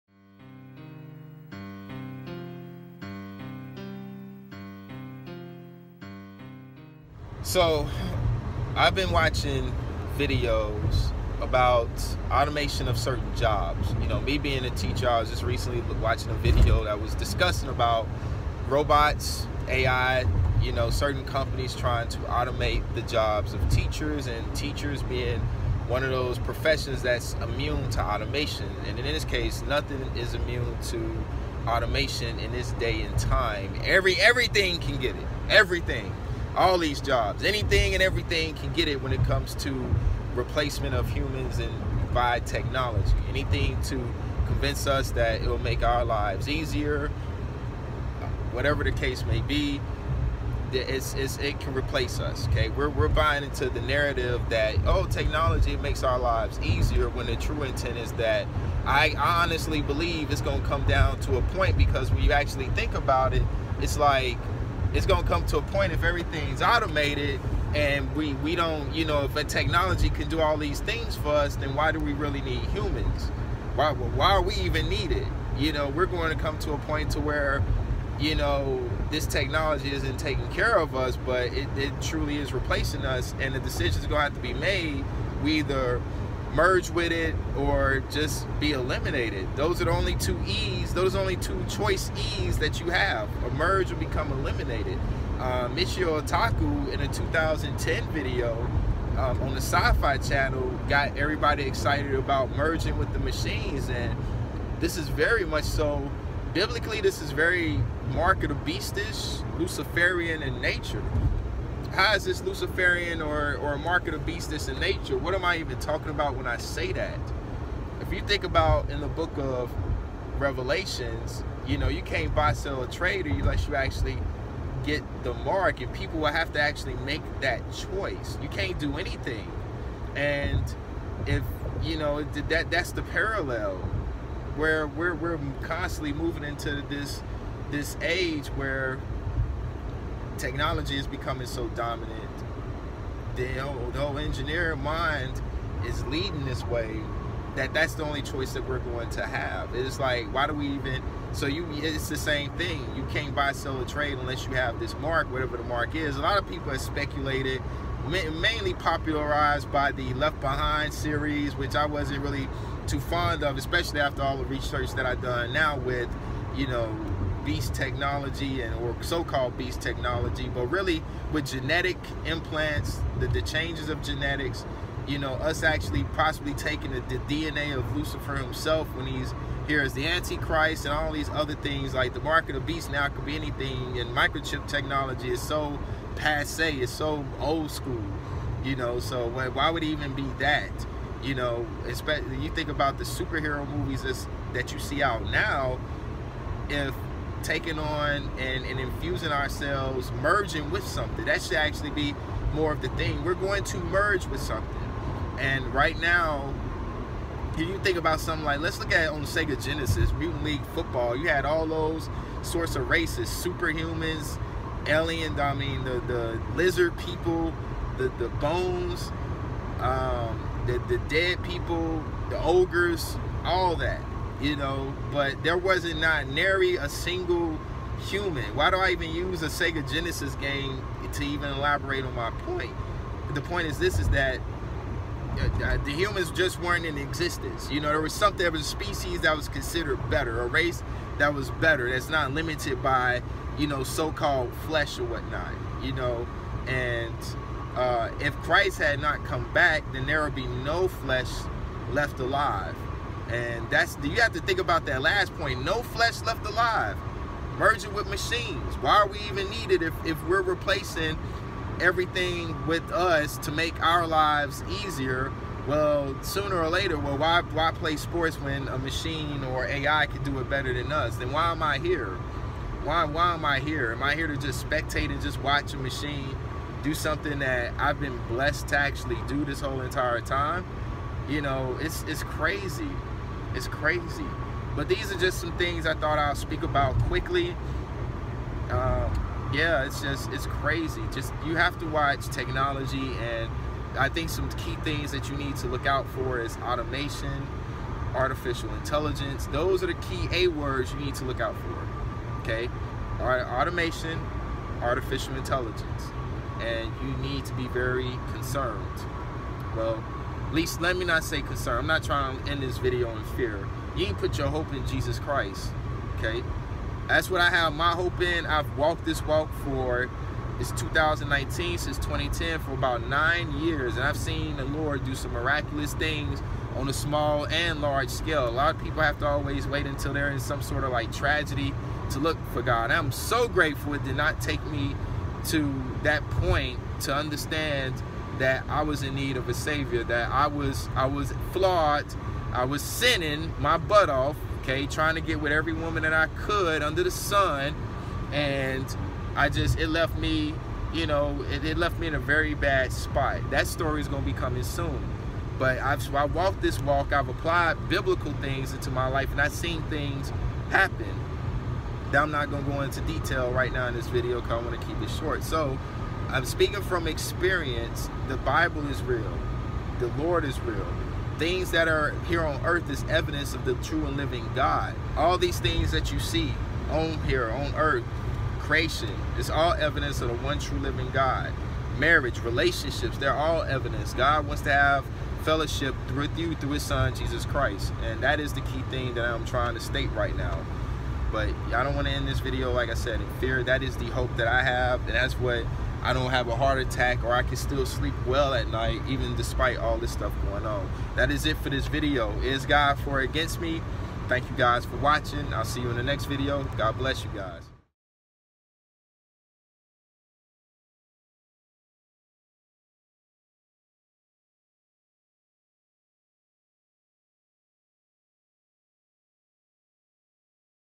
To Man and the Devil, You are Replaceable. To God, You're Irreplaceable!
Will the world need human beings? Who is to say?  Time can only tell, but what it tells us is that we are headed in that direction.  Regardless, the Lord Jesus Christ still and forever will love His creation. The question is will you love Him back or fork over your rights in the name of automation?